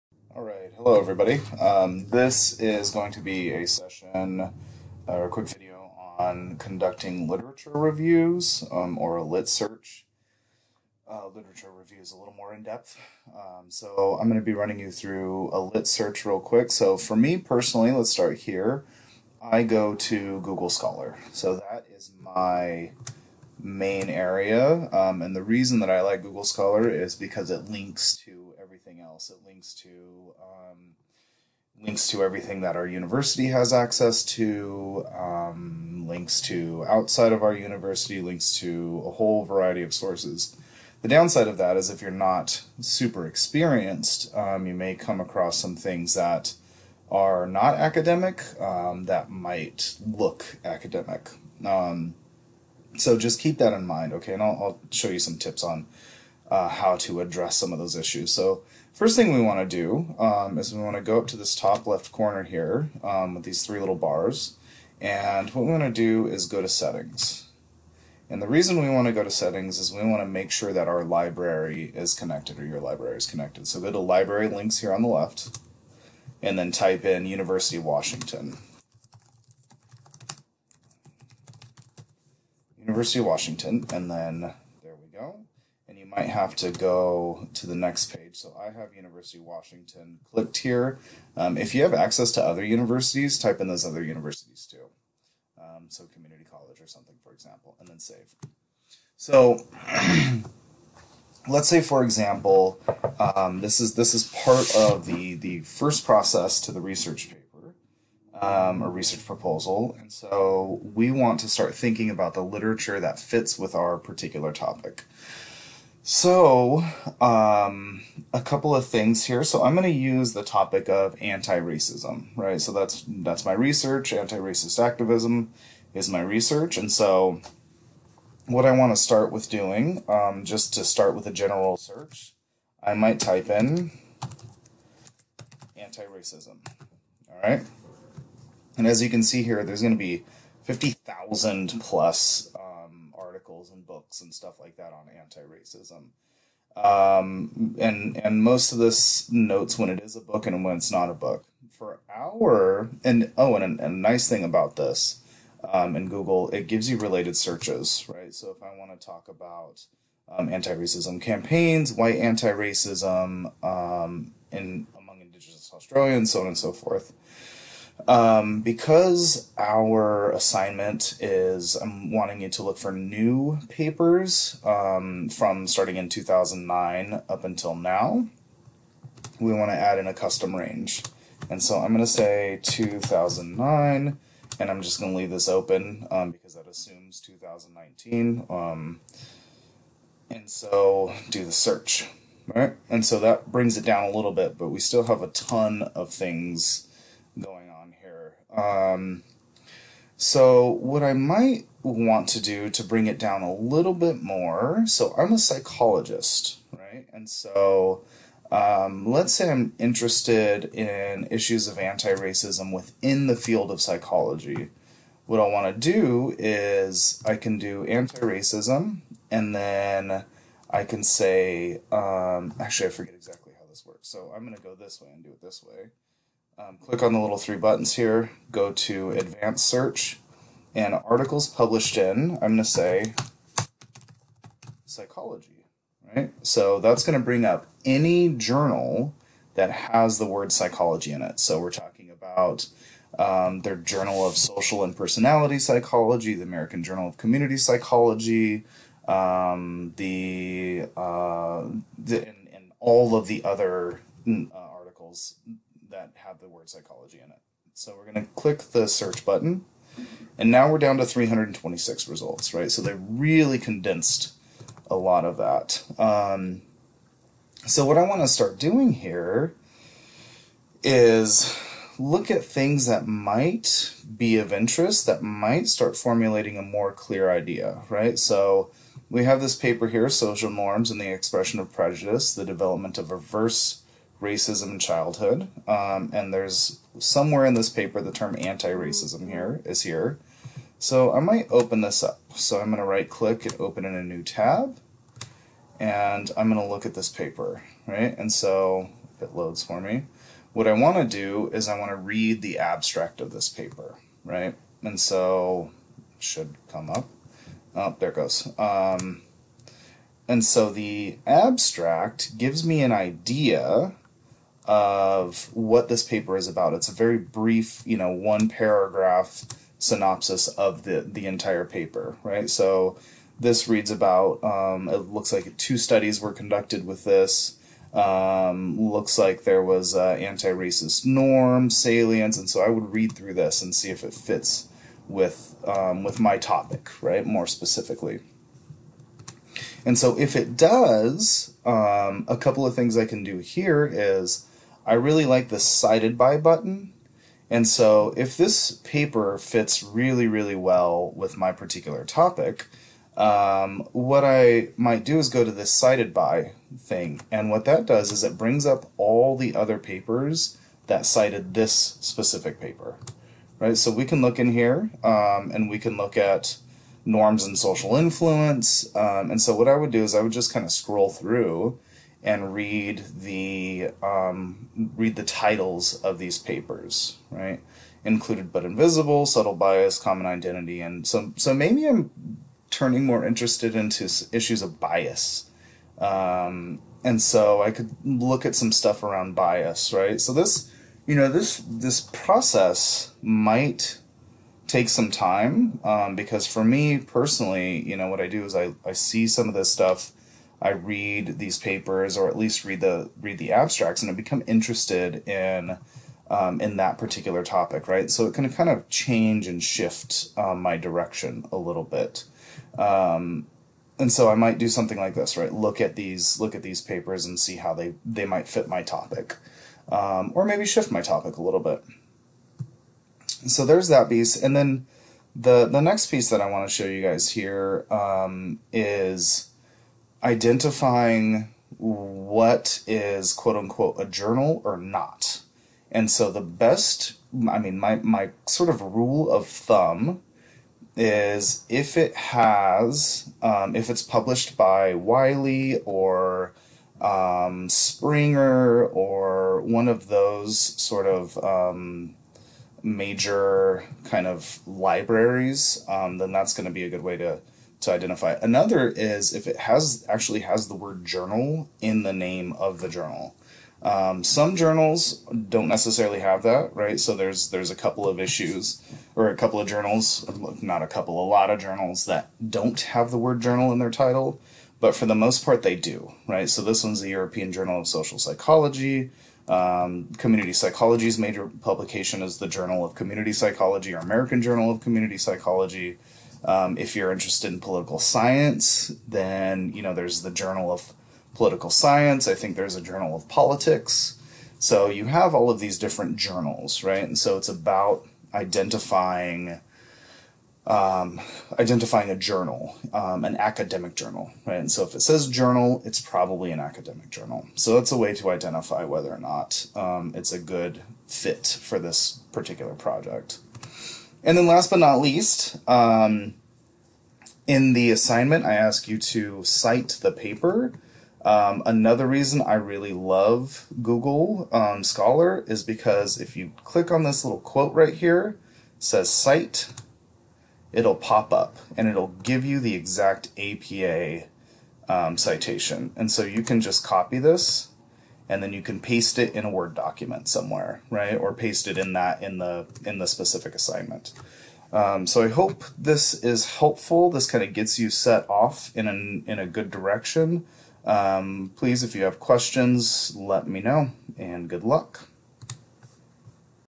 Research Proposal: Lit Review
Instructions on conducting a literature review in Google Scholar